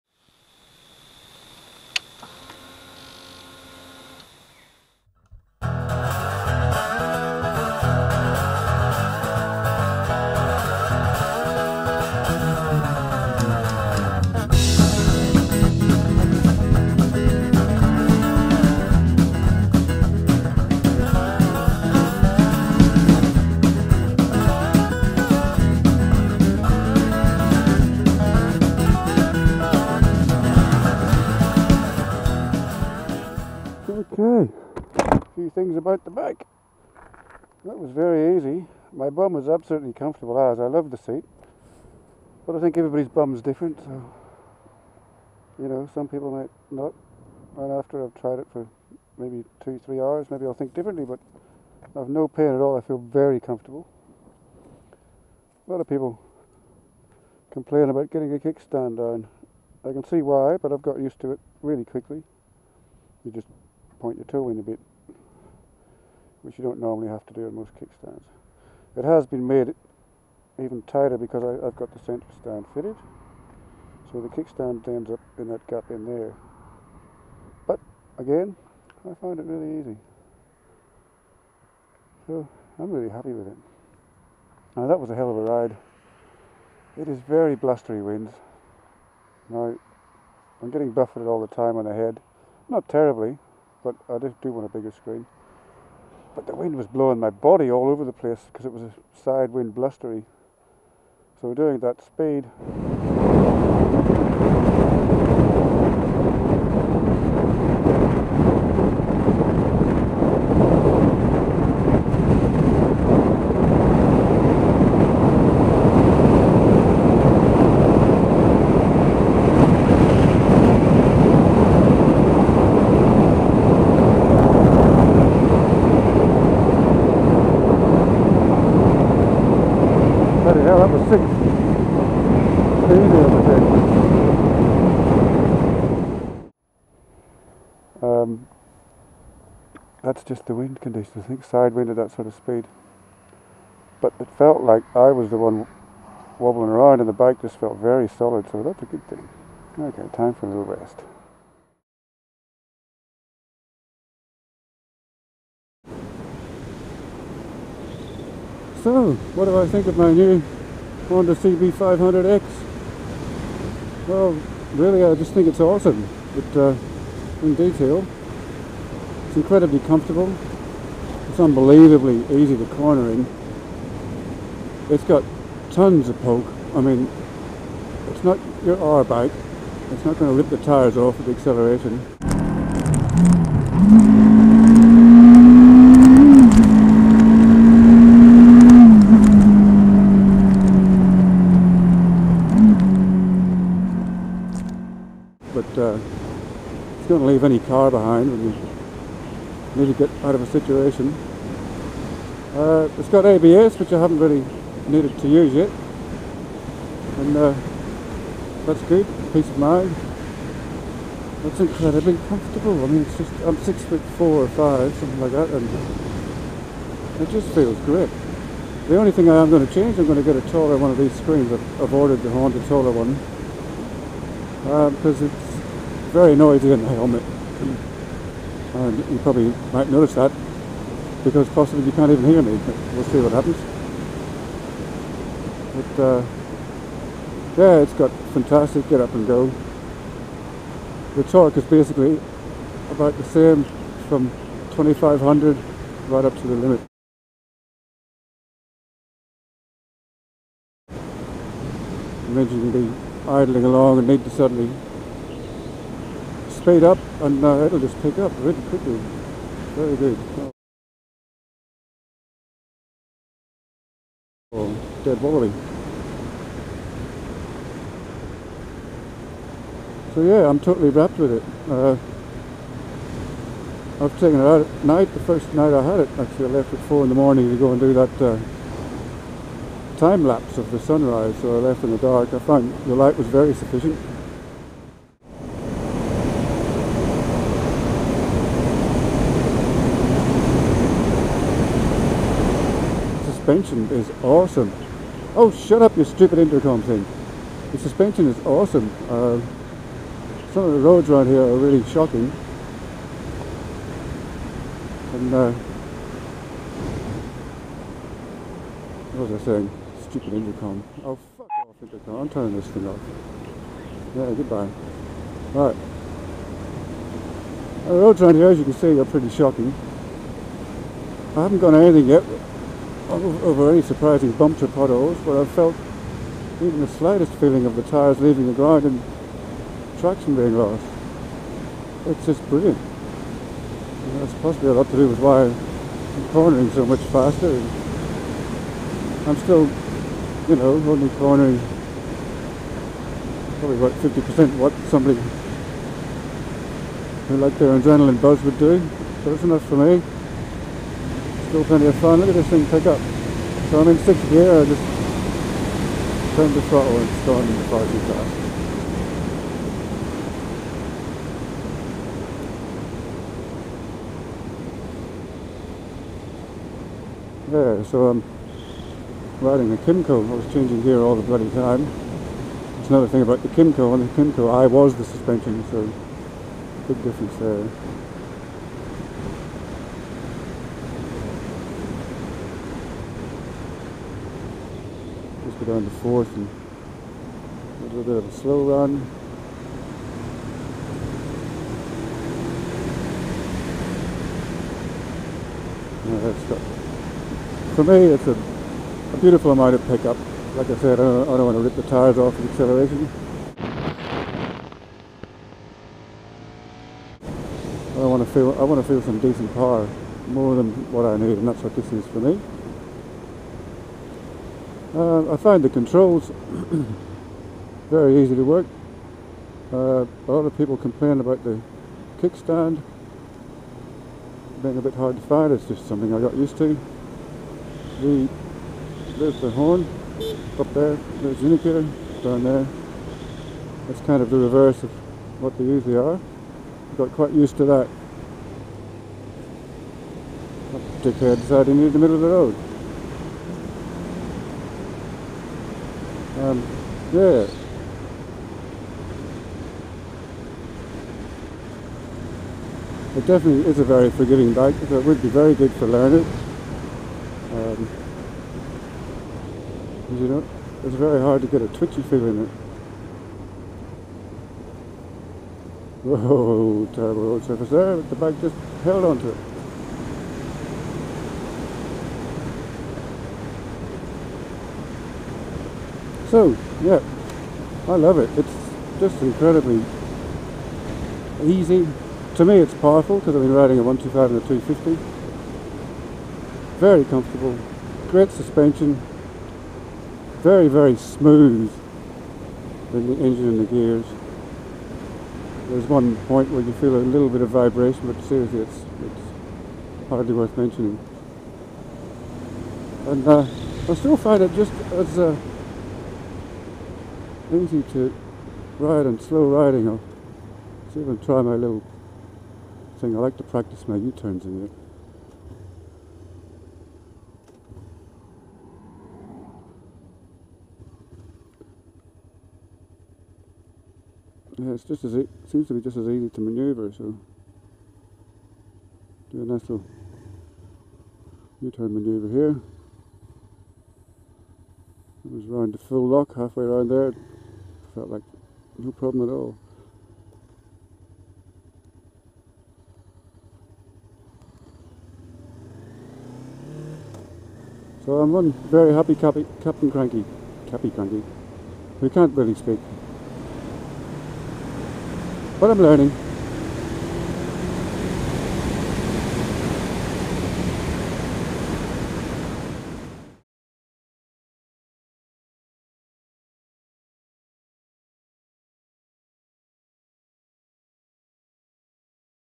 Honda CB500X first review
A few of you guys asked me to review the bike, I try to say more than "it's awesome" but really - it's AWESOME!